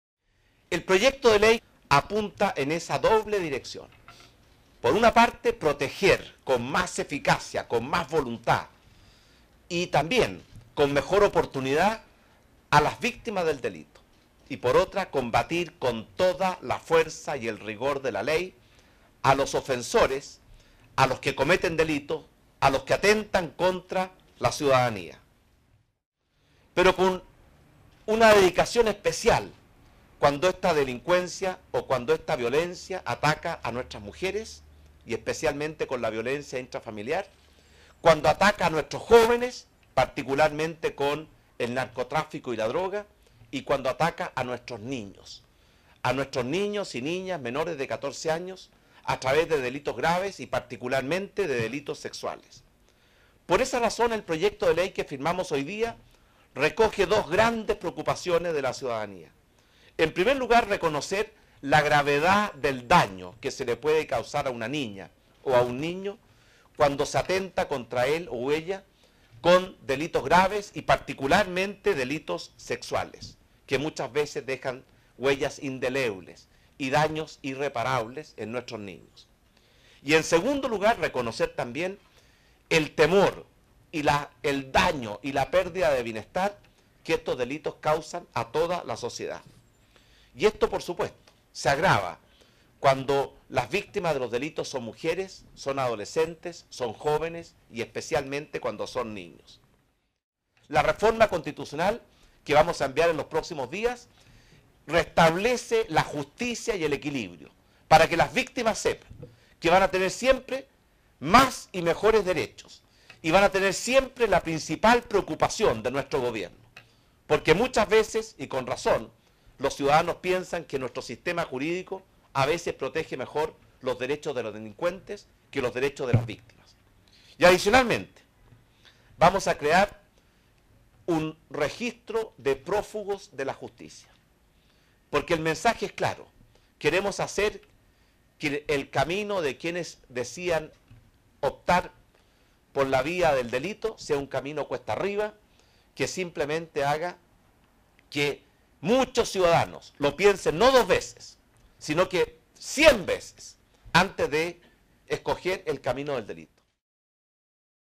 El mensaje es muy claro, nuestro gobierno estará siempre del lado de las víctimas
El Jefe de Estado envío esta mañana al Congreso un proyecto de ley que establece inhabilidades para quienes sean condenados por delitos sexuales contra menores y que crea, además, un registro nacional de condenas por estos delitos.

Asimismo, anunció el envío de una reforma constitucional que permitirá mejorar la defensa de las víctimas y la creación de un registro de prófugos de la justicia